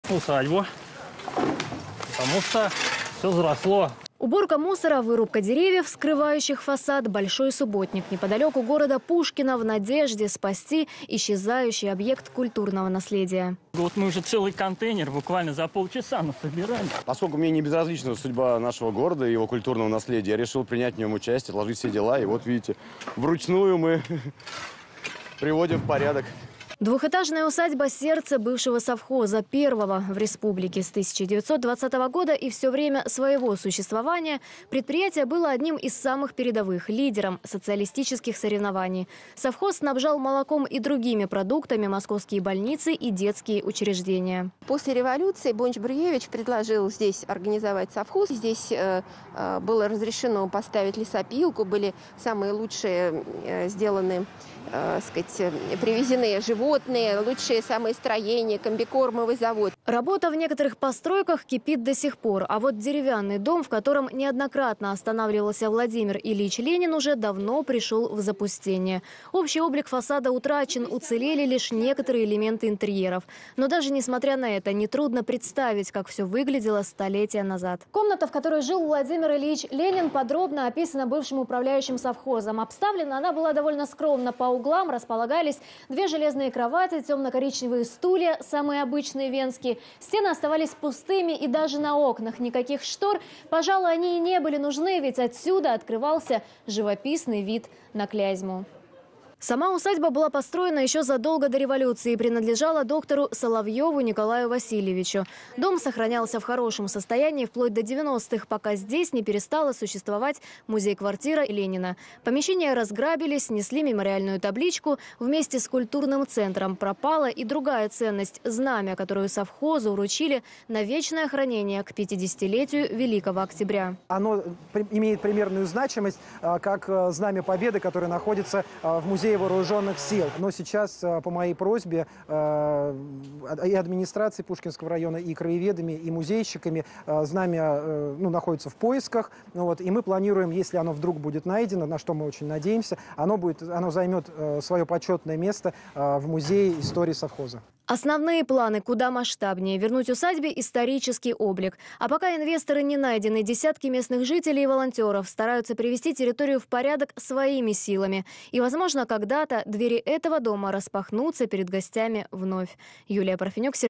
Волонтеры расчистили усадьбу первого совхоза
Волонтеры расчистили усадьбу первого совхоза 26 09 20
Помощь каналу.